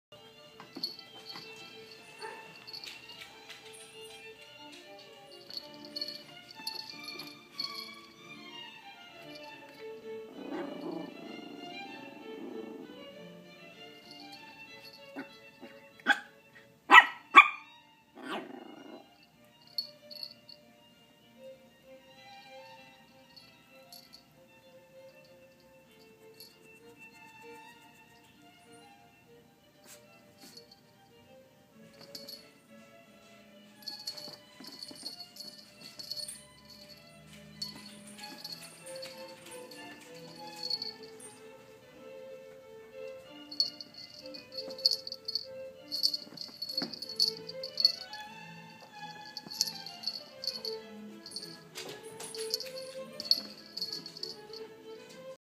Play with me #2
Kitten and puppy play well